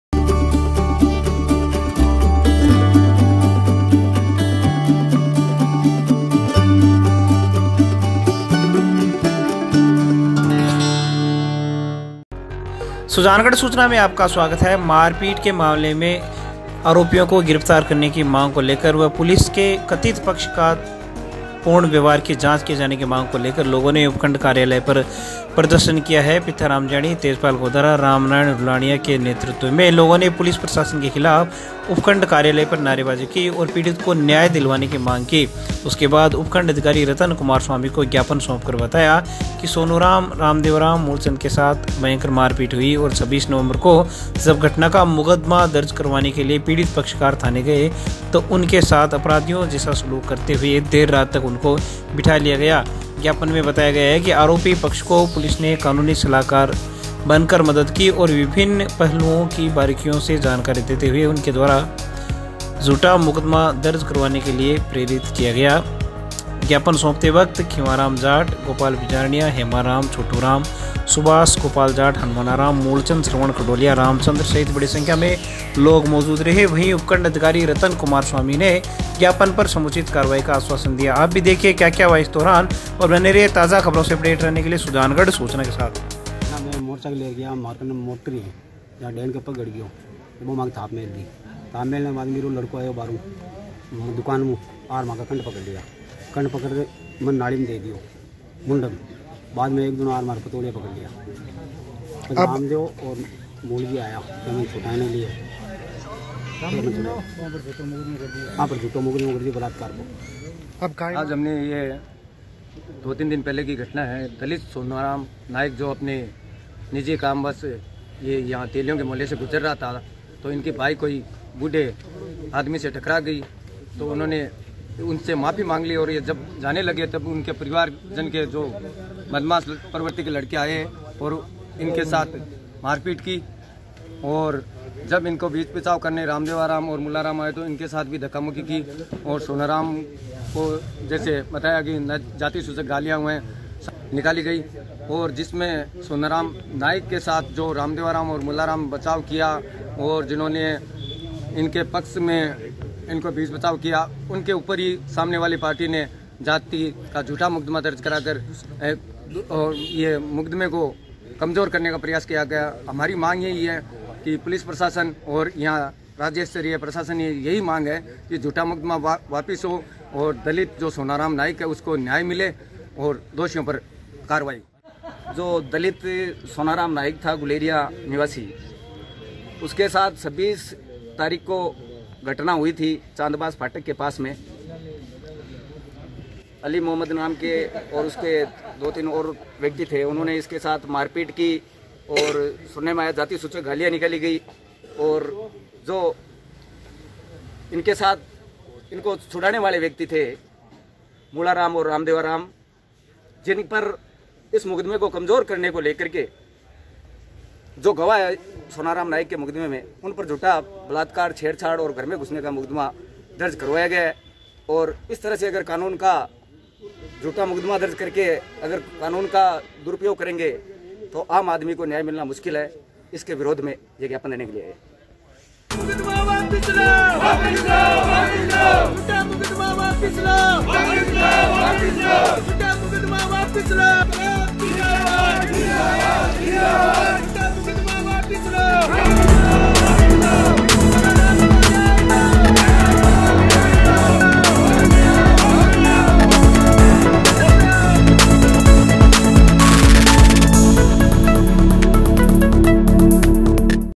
सुजानगढ़ उपखंड कार्यालय पर क्यों हुआ प्रदर्शन? 29-11-19
सुजानगढ़ उपखंड कार्यालय पर क्यों हुआ प्रदर्शन? देखिये सुजानगढ़ सूचना पर